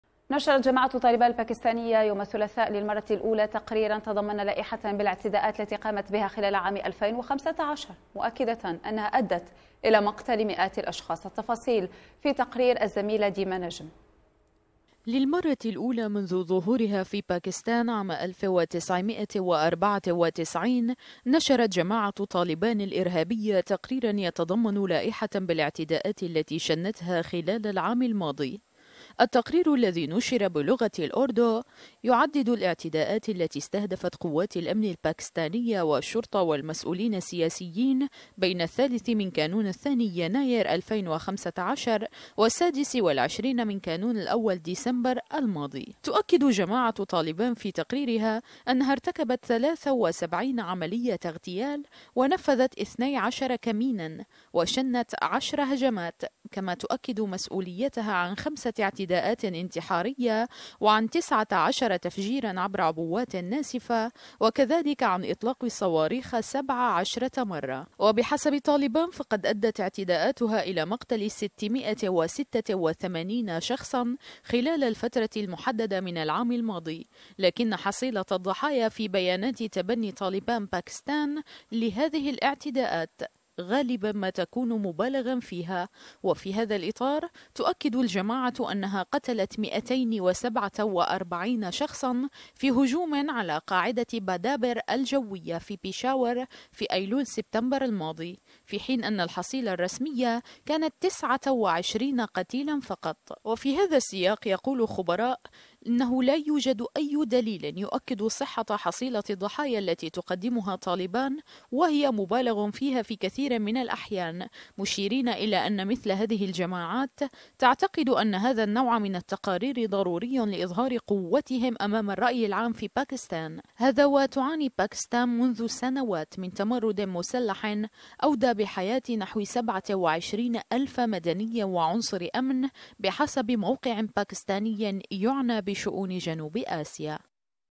طالبان باكستان تنشر لائحة باعتداءاتها خلال عام 2015
نشرت جماعة طالبان الباكستانية الثلاثاء للمرة الاولى تقريرا تضمن لائحة بالاعتداءات التي قامت بها خلال عام 2015، مؤكدة انها ادت الى مقتل مئات الاشخاص.

للمرة الاولى منذ ظهورها في باكستان عام ألف وتسعمئة وأربعة وتسعين، نشرت جماعة طالبان الإرهابية تقريرا يتضمن لائحة بالاعتداءات التي شنتها خلال العام الماضي.

التقرير الذي نشر بلغة الاوردو، يعدد الاعتداءات التي استهدفت قوات الامن الباكستانية والشرطة والمسؤولين السياسيين بين الثالث من كانون الثاني/يناير 2015 والسادس والعشرين من كانون الاول/ديسمبر الماضي.